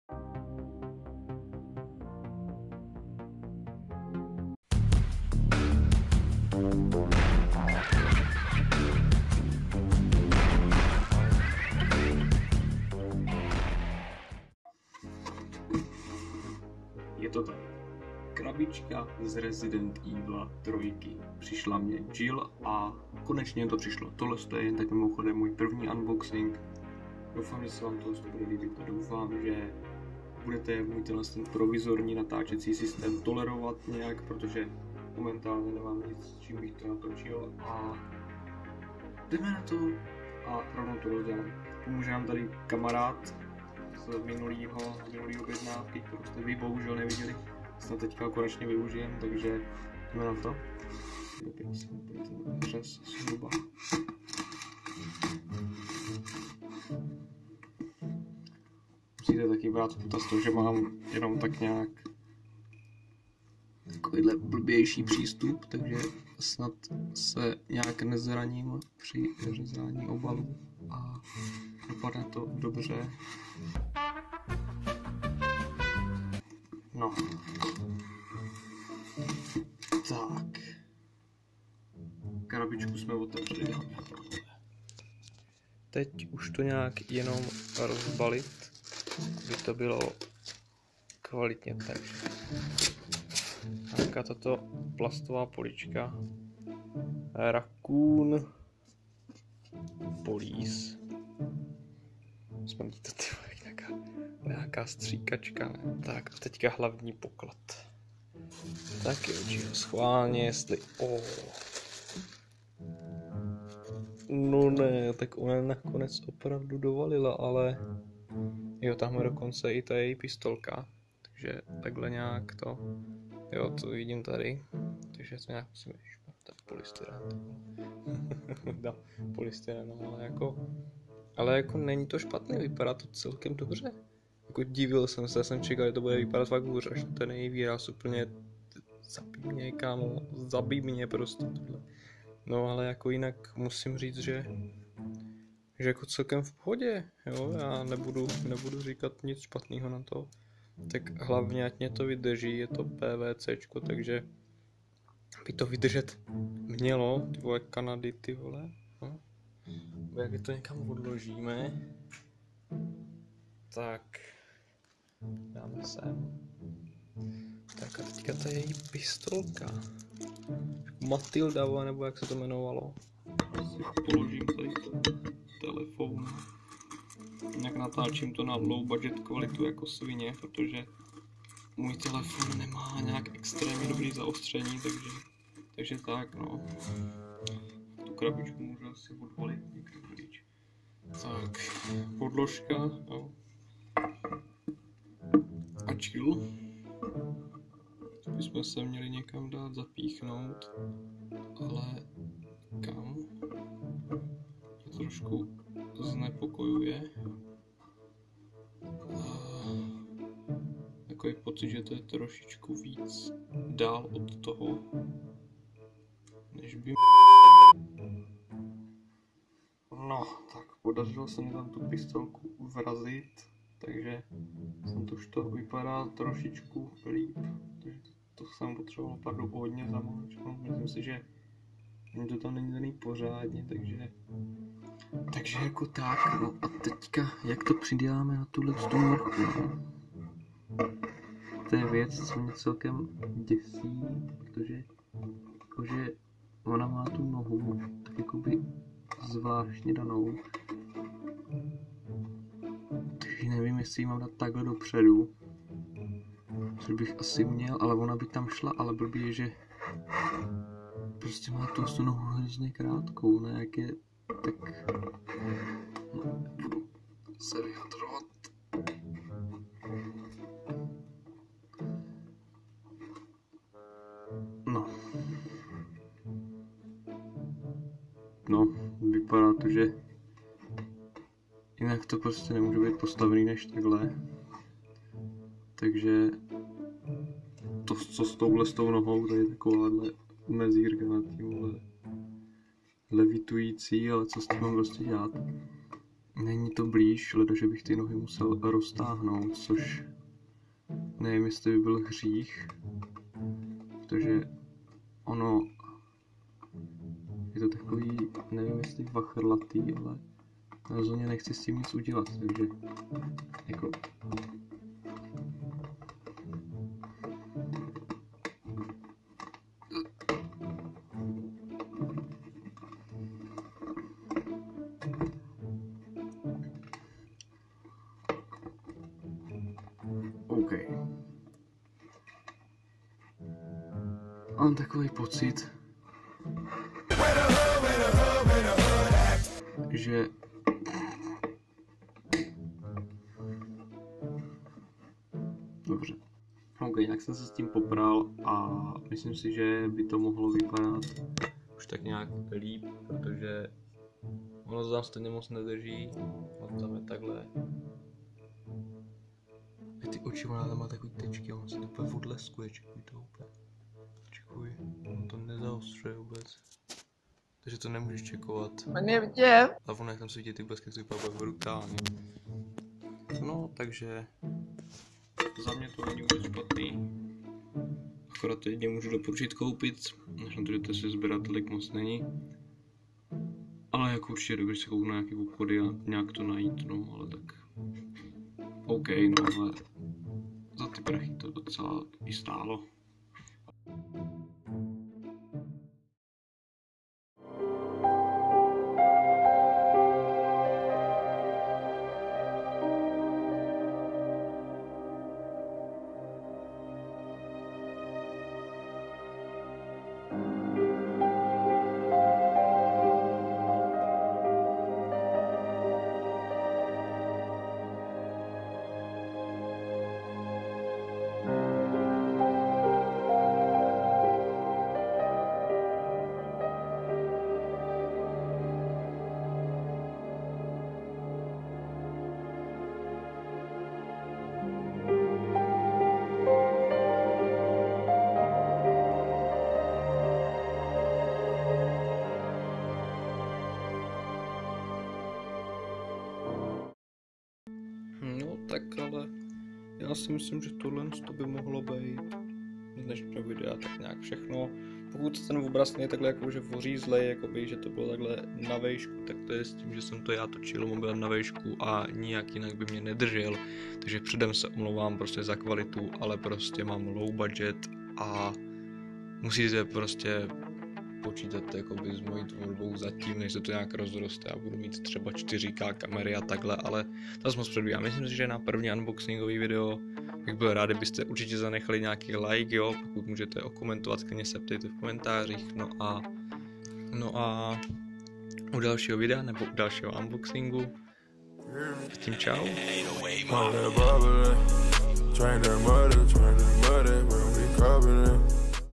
Můj první unboxing na RE3 remake - Jill
Ahoj lidi. Vítám vás mým prvním videem v roce 2021.